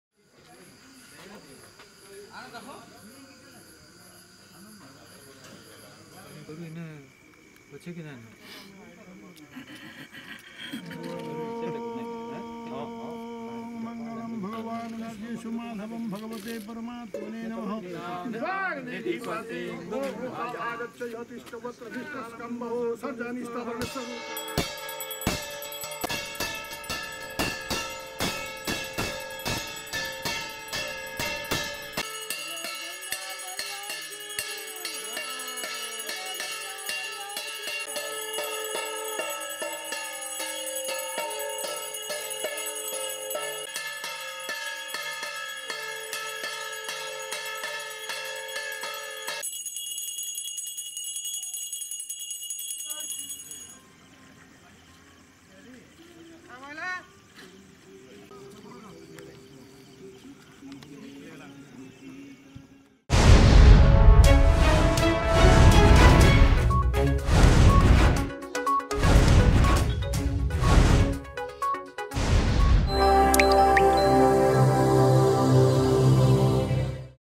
ତ୍ରିବେଣୀ ସଙ୍ଗମରେ ମହା ଆଳତୀ....
ସୁବର୍ଣ୍ଣପୁର ସହରର ପ୍ରାଚୀନ ଶୈବପୀଠ ରାମେଶ୍ବର ମନ୍ଦିର ପାର୍ଶ୍ବ ତ୍ରିବେଣୀ ସଙ୍ଗମରେ ମହା ଆଳତୀ.